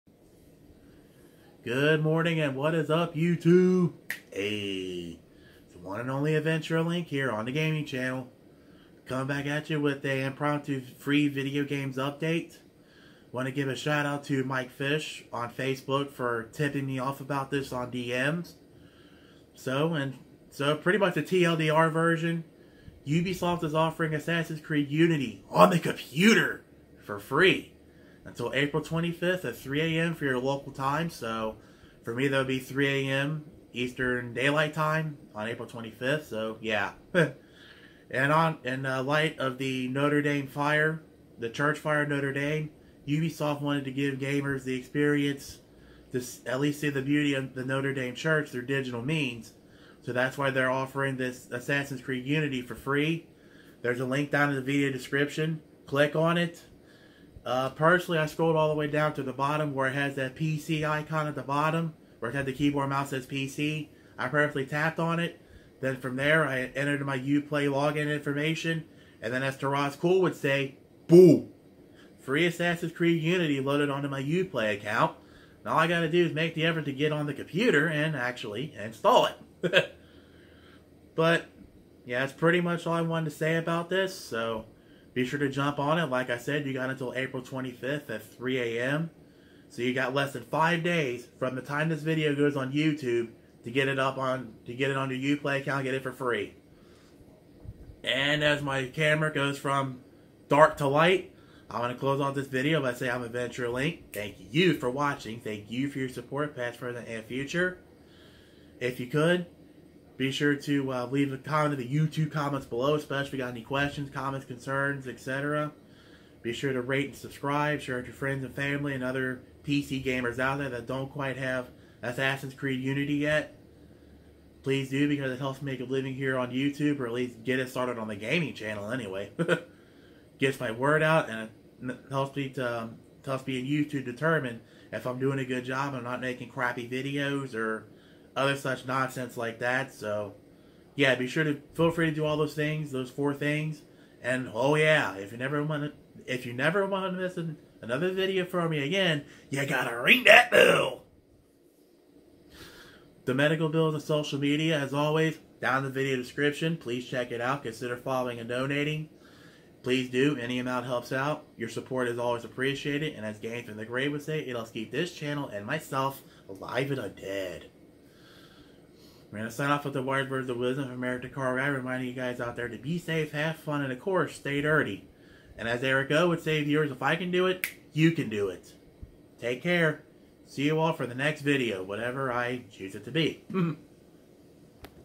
Impromptu free game alert: Assassin's Creed: Unity for free on PC till 25 Apr 2019 @ your local time
So earlier this morning, I was tipped off by my friend Mike Fish on Facebook in my DMs about Assassin's Creed: Unity being free on the computer!

In light of the Notre Dame de Paris Cathedral fire, Ubisoft wants to give gamers the chance to see the Notre Dame Cathedral in its digital glory, therefore they are offering AC:U for free until 25 April @ 3am, depends on your local time.